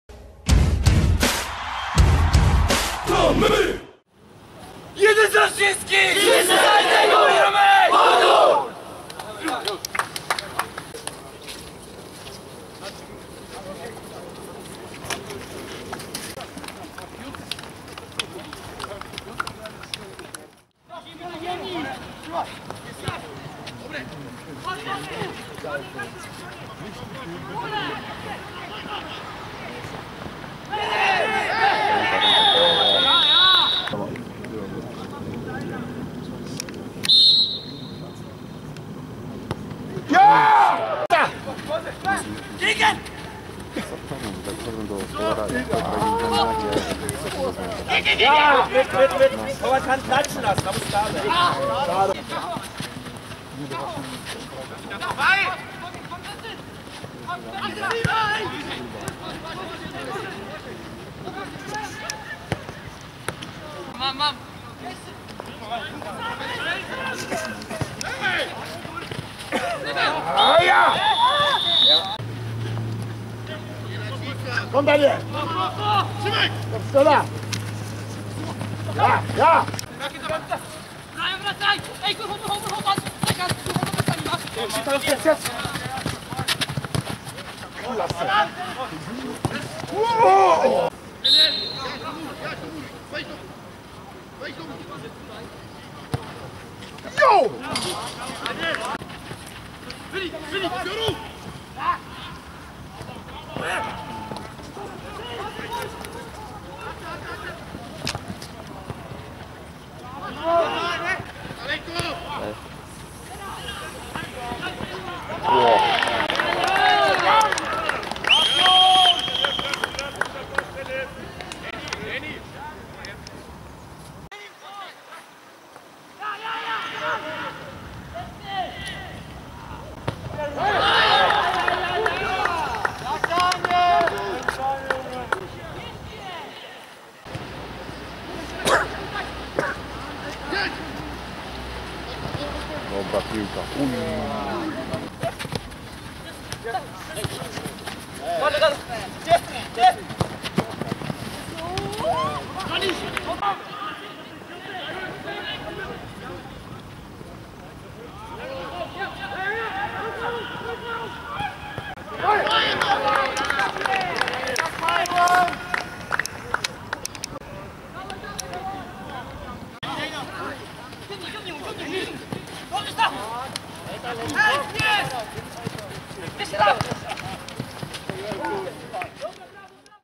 Sparing: Union Berlin - Pogoń Szczecin (JM) 3:0 (0:0)
W niedzielę podopieczni Piotra Łęczyńskiego pojechali do Berlina, gdzie walczyli z Unionem. W pierwszym sparingu w tym roku przegrali 0:3.

sparing
Union Berlin - juniorzy młodsi Pogoni Szczecin 3:0 (0:0)

Bramki:
1:0 - Lucas Brumme - 50'
2:0 - Daniel Eidtner - 51'
3:0 - Ufuk Korkut - 66'

Pogoń Szczecin: Daniel Kusztan (58' Kacper Pietrzak) - Kacper Plebańczyk (40' Kacper Michalski), Szymon Jopek, Wojciech Błyszko (62' Hubert Sadowski), Daniel Rotengruber - Stanisław Wawrzynowicz, Błażej Starzycki (51' Michał Graczyk), Jan Imiołek (62' Mateusz Duchowski) - Bartosz Boniecki (51' Bartłomiej Białek), Dominik Nowakowski - Adrian Benedyczak.